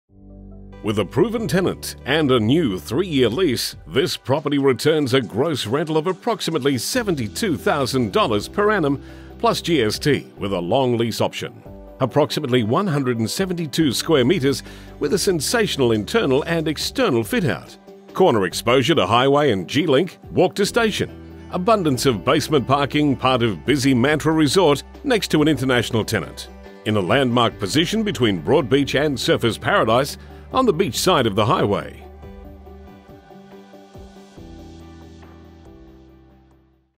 Lot 9 2893 Gold Coast Highway Surfers Paradise 4217 QLD by D...
With a proven tenant and a new three year lease this property returns a gross rental of approximately Seventy Two thousand dollars ($72K) per annum plus GST with a long lease option.Approximately One hundred and seventy two square metres with a sensational internal and external fit out.Corner exposure to highway &amp; G-Link - walk to station. Abundance of basement parking, part of busy Mantra Resort next to international tenant.In a landmark position between Broadbeach &amp; Surfers Paradise on Beachside of highway.
9/2893 Gold Coast Highway, Surfers Paradise